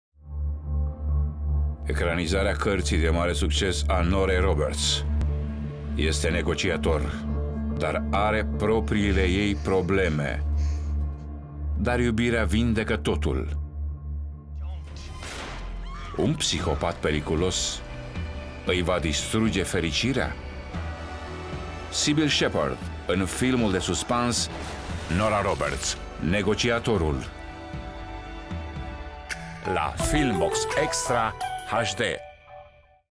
NORA ROBERTS - NEGOCIATORUL
Experta negociatoare Phoebe McNamara se împarte între cariera ei stresantă, creşterea fetiţei şi îngrijirea mamei agorafobe, Essie.